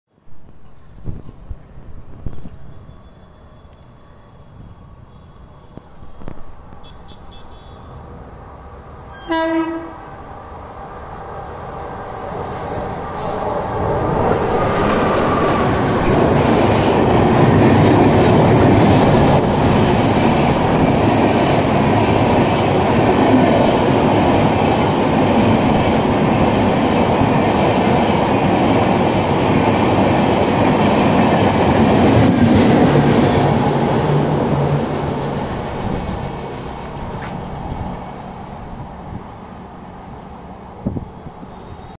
😳1st Lhbfied Run 22160 MAS- CSMT Mumbai express. | BSL WAP4 ♥️ | Indian Railways 🇮🇳
Mgr Chennai Central - Mumbai csmt Superfast SF Express on 1st Lhbfied Run from Mas today Skipping PEW Sunny Afternoon

Loco: Bhusawal WAP4 #22952

Train Number:22160/59

Coach type: Red Lhbfied Rake

🇮🇳Jai Hind Indian Railways 🧡🤍💚

😍Lhb + P'4 emotion ♥️